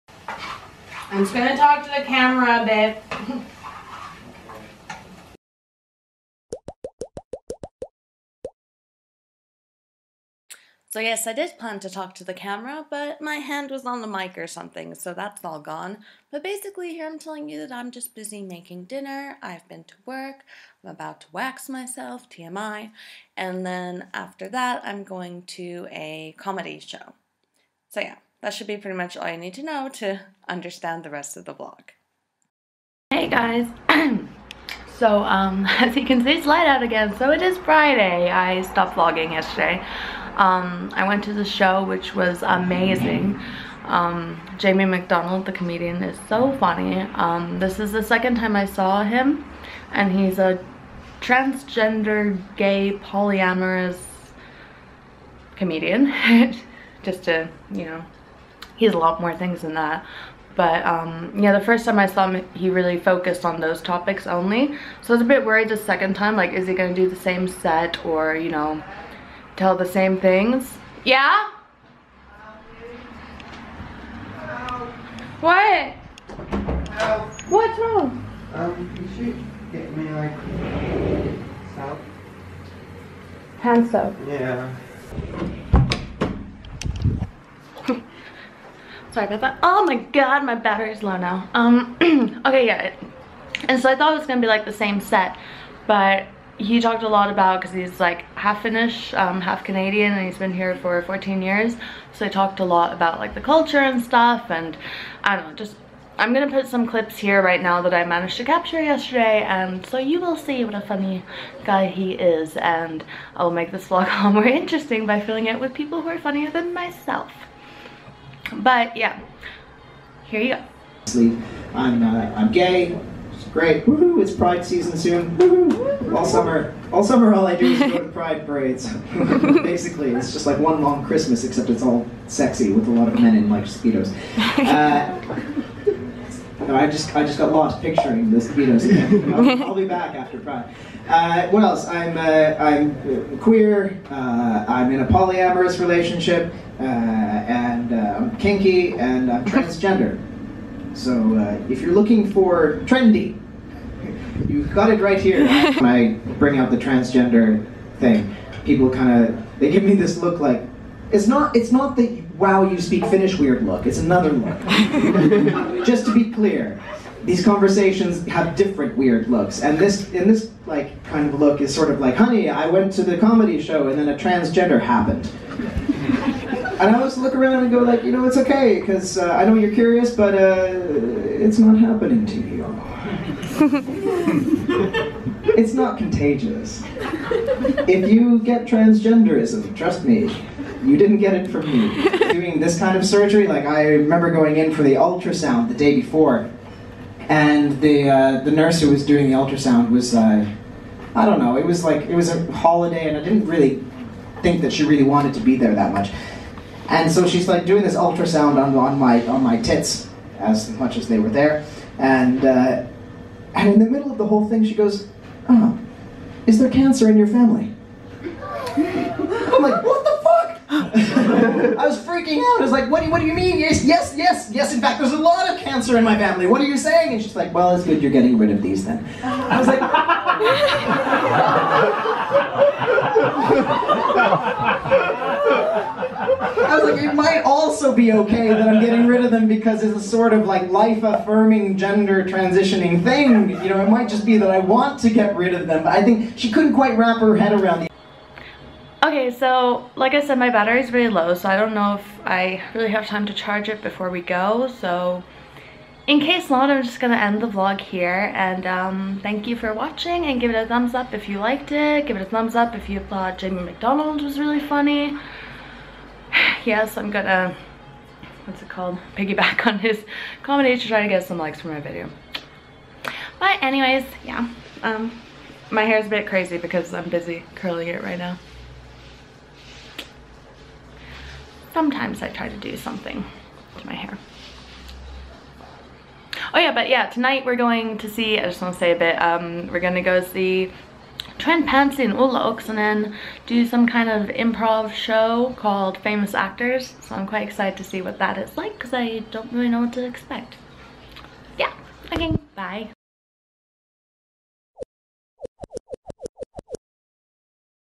Ep27: Comedy shows and... that's it.
Episode 27: In which... let's be honest, Anna's performance is not very great. She covers the mic with her hand, she forgets to close the vlog and in general is a little scatter brained :D.. BUT despite this lackluster performance, the vlog is saved by Jamie MacDonald! Super funny comedian who's clips steal the show.. or are the show :)